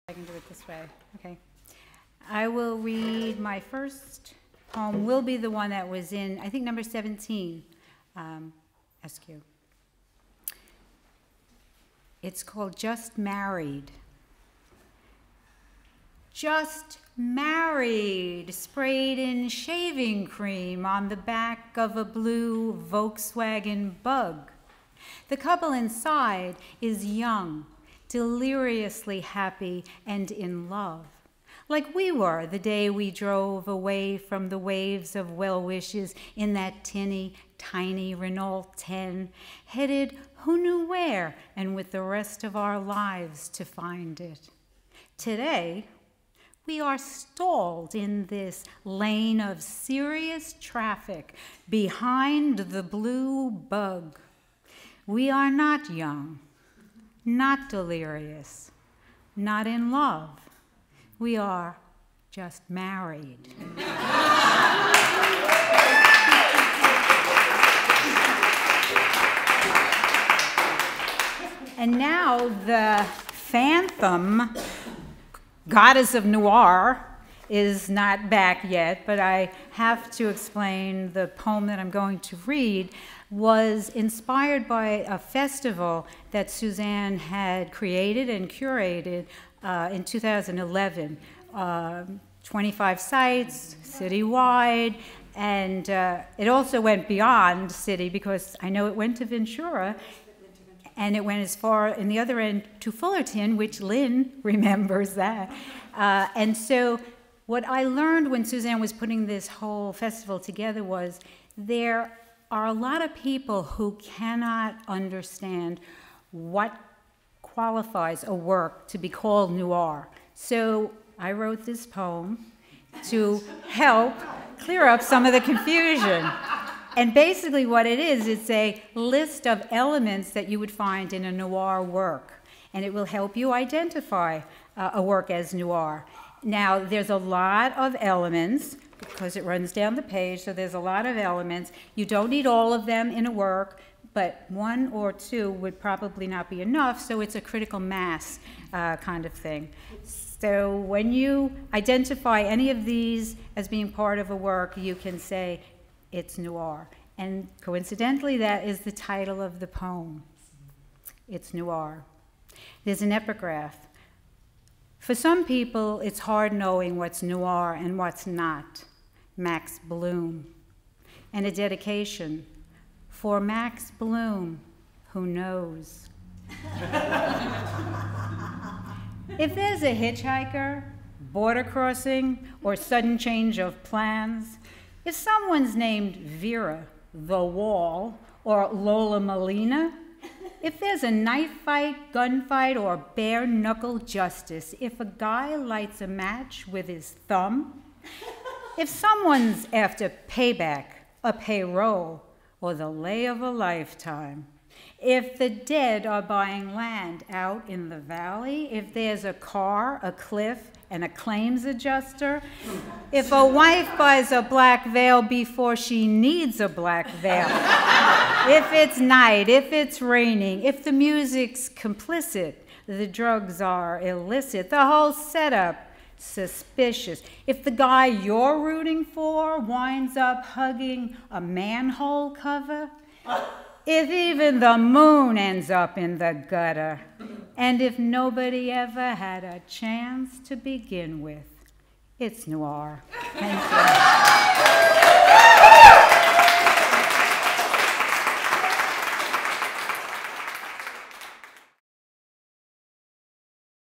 Cece Peri, Askew poetry reading at Ave. 50
Ceci Peri reads her poems, Just Married and It's Noir at Ave. 50 Studios in Highland Park (Los Angeles), California on March 29, 2016.

This reading was to celebrate the publication of Askew #18.  Thank you, Jessica Ceballos for inviting us to read.

                                                          video by Phil Taggart